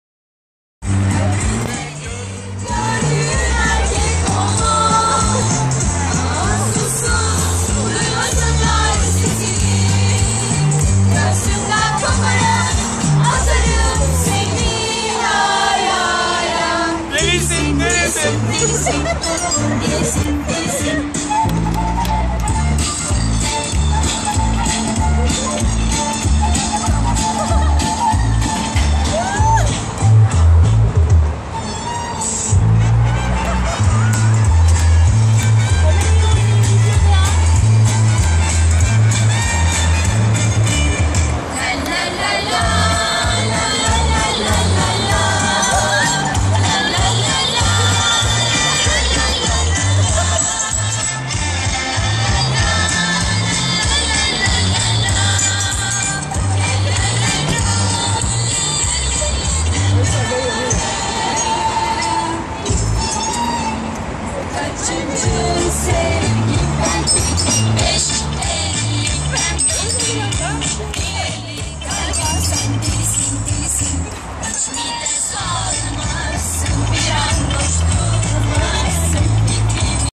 Kiralık Gelin Arabası Vosvos
Elif & Yusuf çiftimizin ataşehir botanik'te fotoğraf çekim etkinliğinden ufak eğlenceli bir kesiti. videodaki vosvos Türkiyenin İLK ve En büyük vosvos kiralama sitesi olan 'dan kiralanabilir